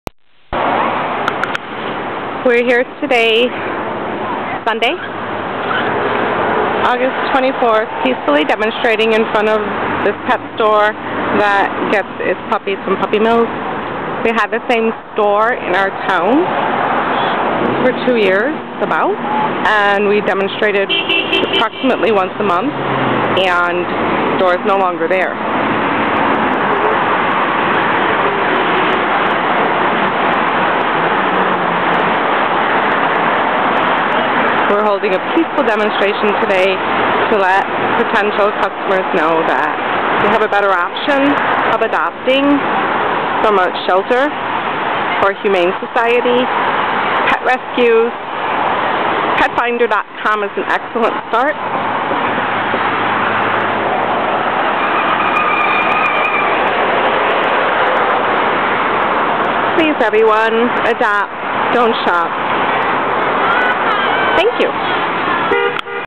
Demonstration in Lombard
Successful demonstration in Lombard today, Sunday, August 24th.  Many passer-by's and potential and past customers stopped to talk with us.  A young couple who had recently purchased a puppy from a pet store were now facing enormous vet bills due to broken, fragile legs bones of their puppy.  They had not known about the pet store, puppy mill connection, prior to purchasing their pet. Now, sadly, they do.  We were here this very day in an effort to be available to anyone who might need guidance in where to attain a great pet.  We have a number of sources that adopt out dogs, cats and other animal companions on a daily basis.  Many of the demonstrators are animal rescuers, vet techs, shelter workers, etc.  We're just trying to help people make an educated choice.  Adopt, don't shop.